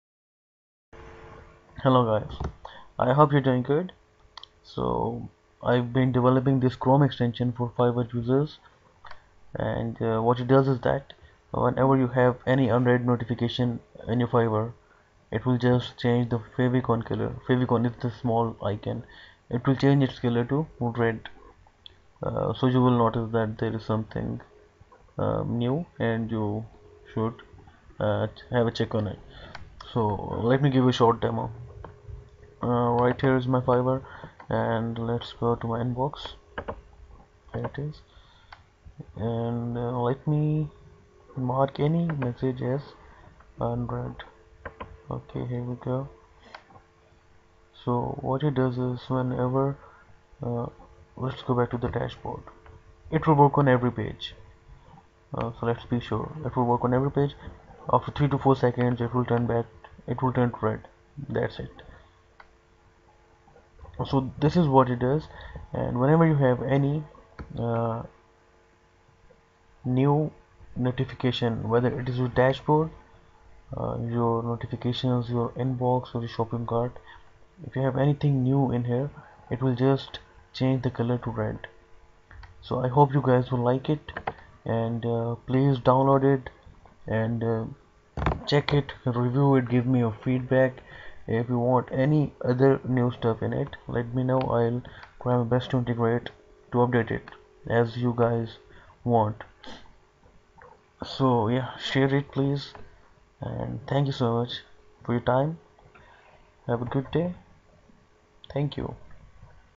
Fiverr Notification Icon in Title - A must have Chrome Extension for Fiverr freelancers
This is a demo of a Chrome Extension "Fiverr Notification Icon in Title".
What it does is that it changes the Fiverr icon on the tab to red whenever you have a new notification so you don't have to check your Fiverr again and again.